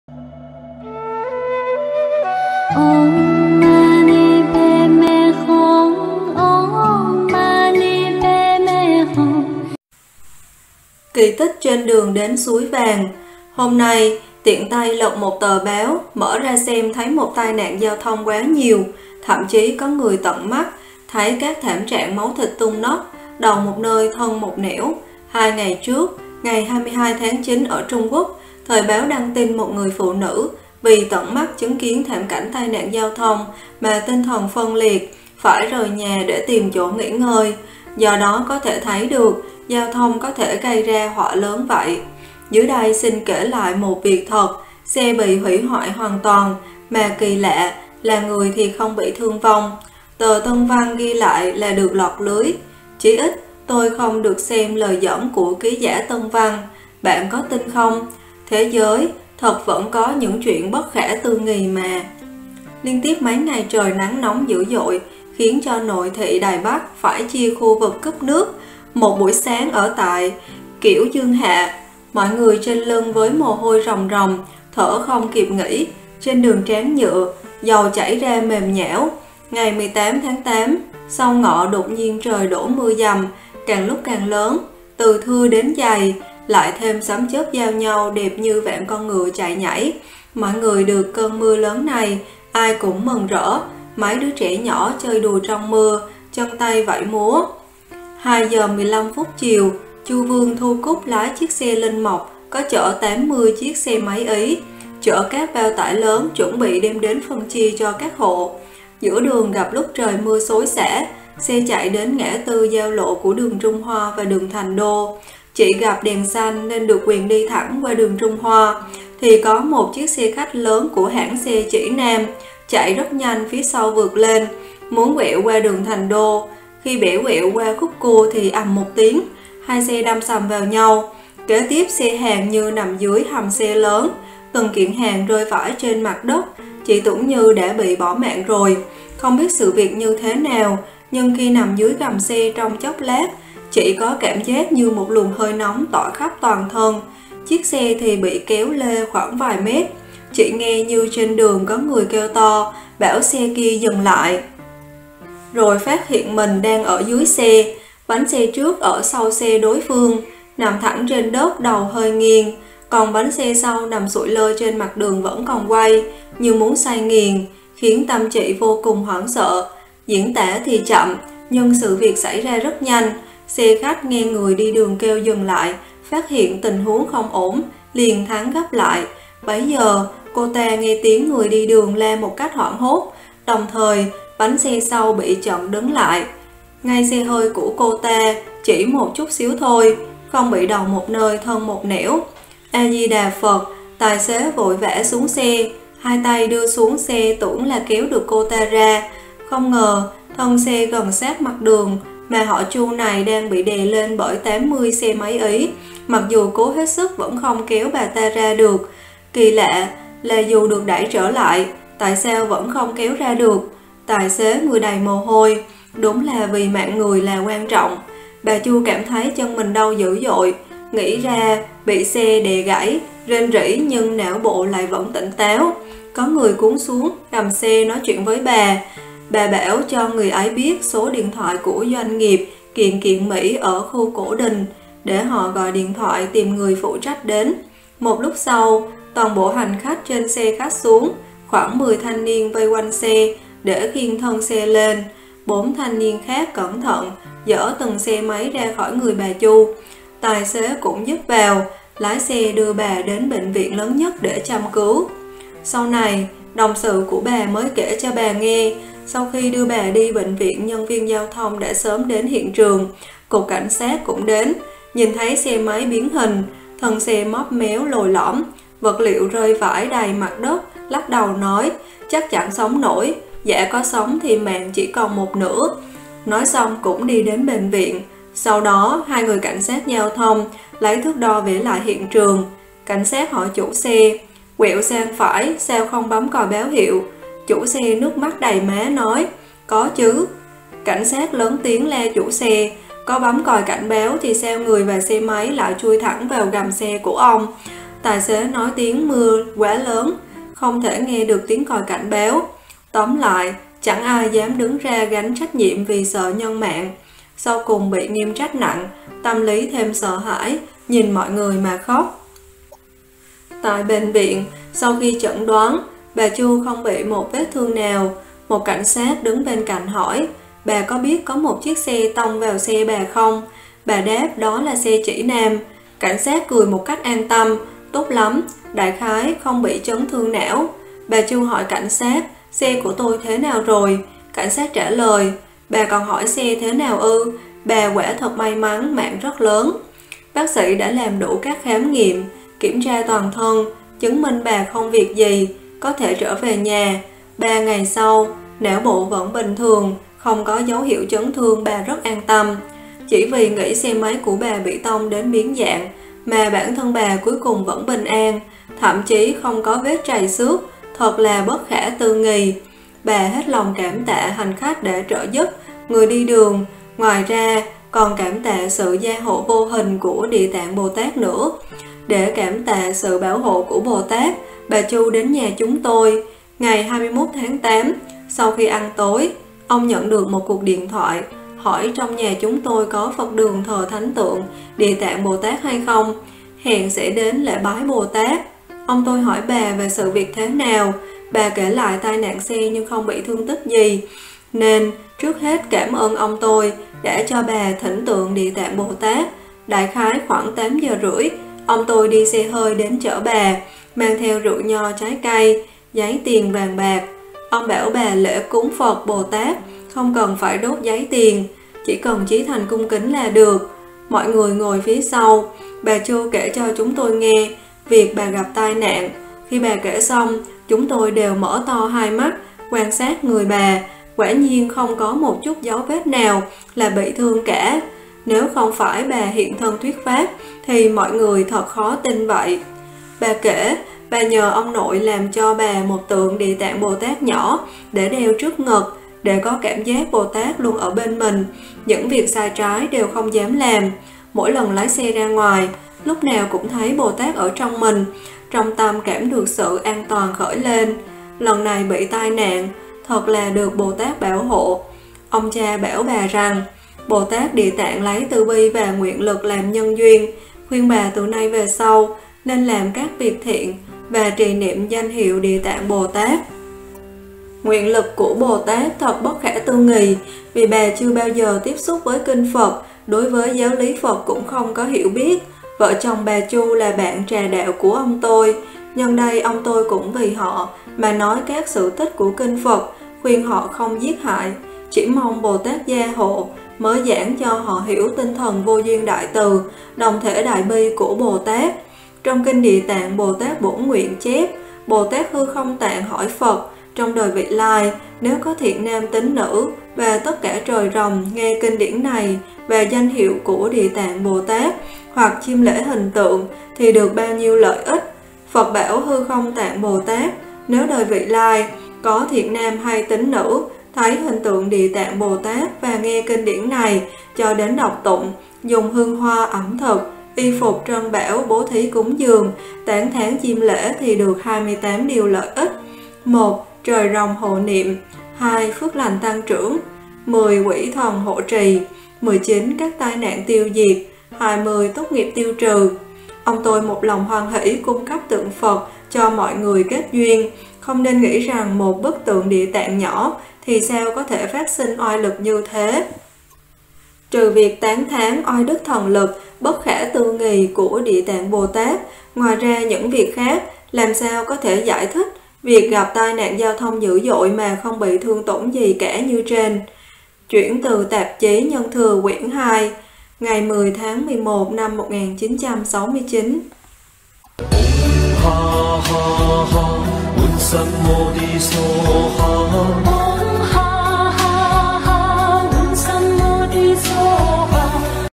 Kỳ tích trên đường đến suối vàng
15 Kỳ tích trên đường đến suối vàng
Trích Phổ khuyến đạo hữu tu Tịnh Độ trì tụng kinh Địa Tạng - Đại Sư Hoằng Nhất
Giọng đọc: Kênh Nhân Quả Luân Hồi
Những Điều lợi ích khi quy y với Địa Tạng Bồ Tát